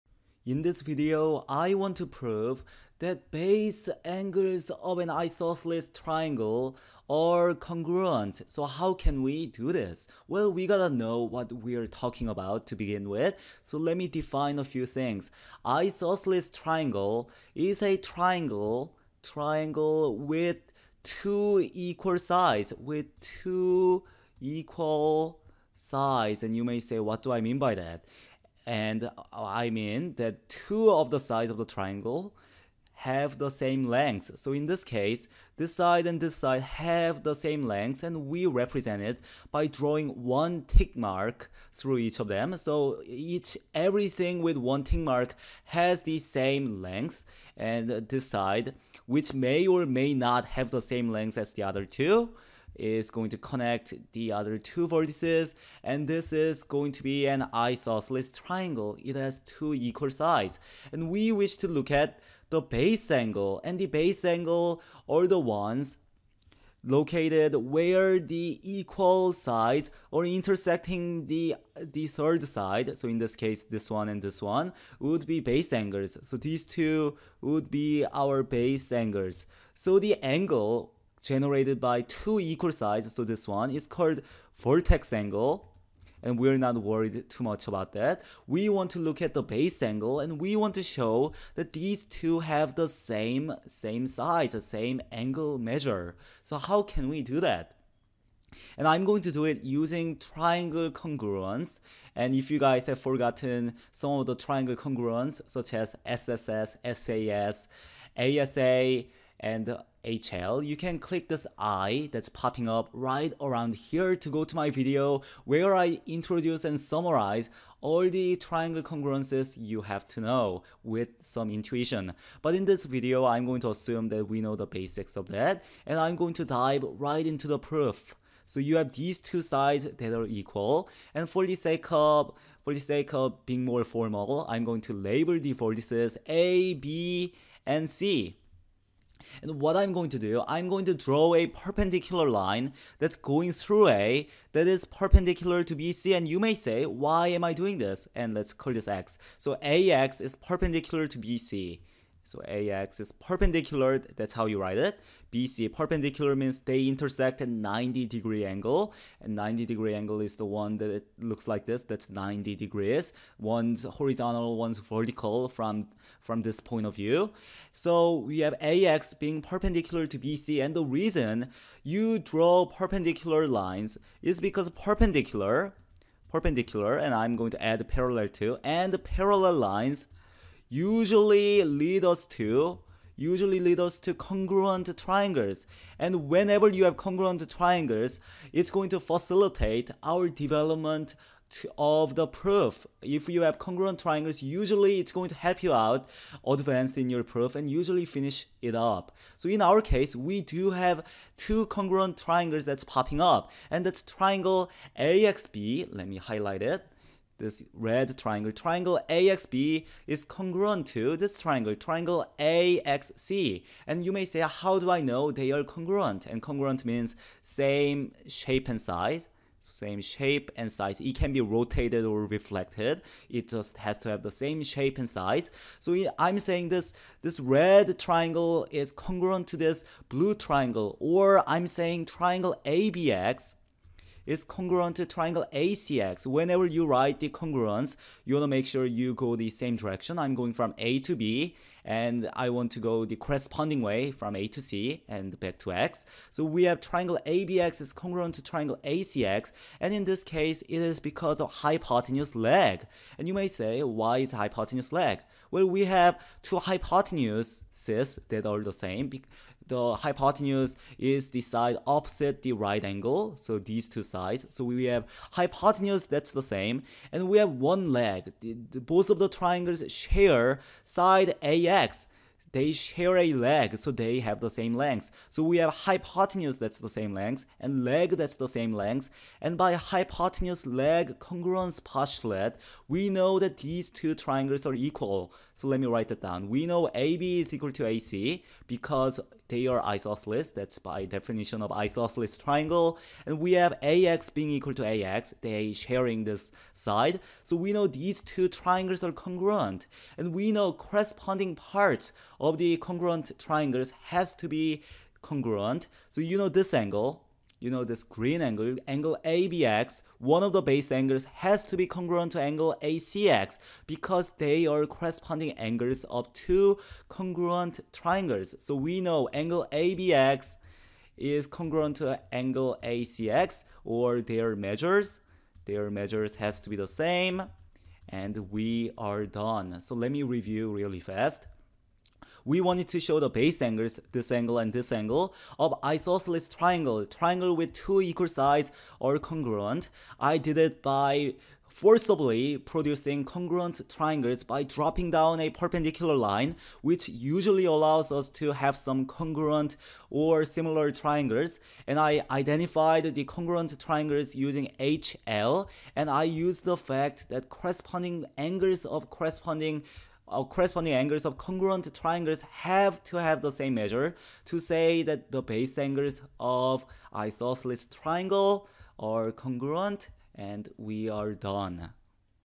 Prove: Base Angles of Isosceles Triangle are Congruent
With an in-depth explanation that includes not only the proof itself but also the intuitive explanations of each step, we prove that the base angles of the isosceles triangle have the same angle measure--they are congruent.